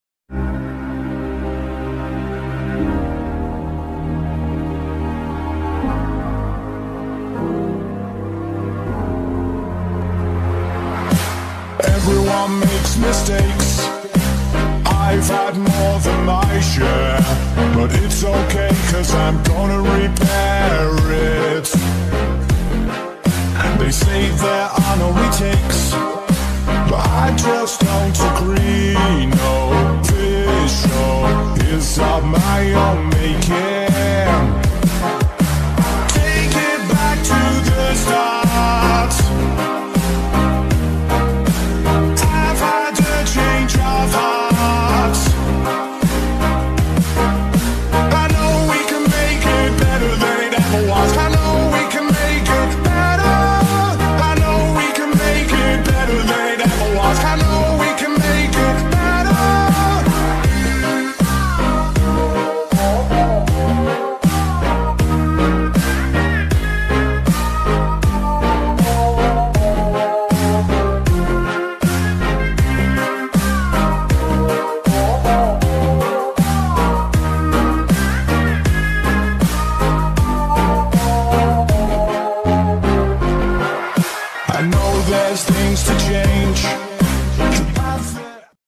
epoch meme(gacha club)leia a descrição
gente esse video e do passado quando aparece eu com meu amigos e aquela pessoa são prints do passado e depois apareçe eu no presente ta bom,tenha um bom video